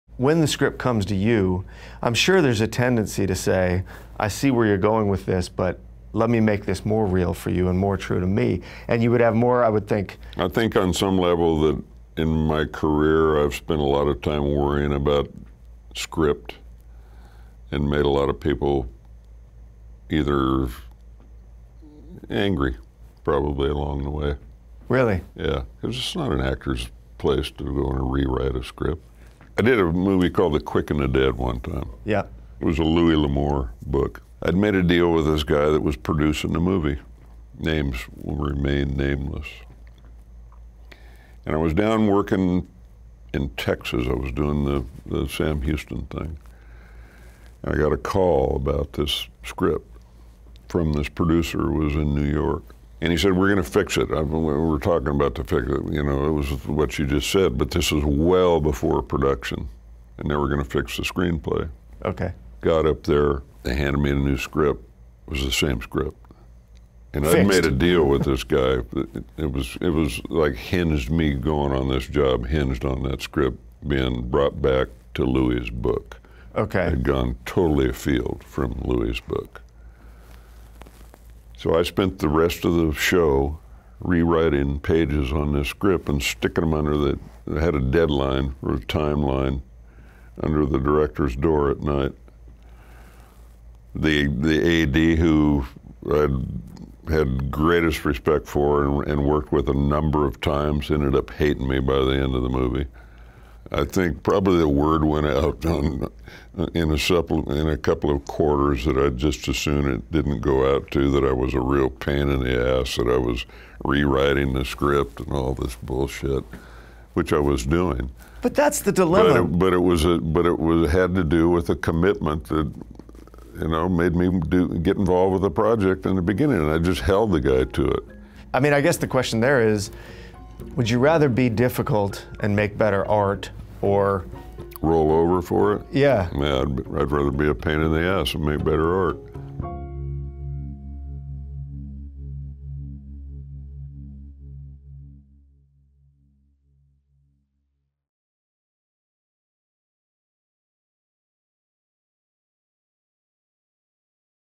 Sam Elliott won't roll over when it comes to making better art
Actor Sam Elliott talks to Sam Jones about his experience being the "pain in the ass" actor who rewrites scripts.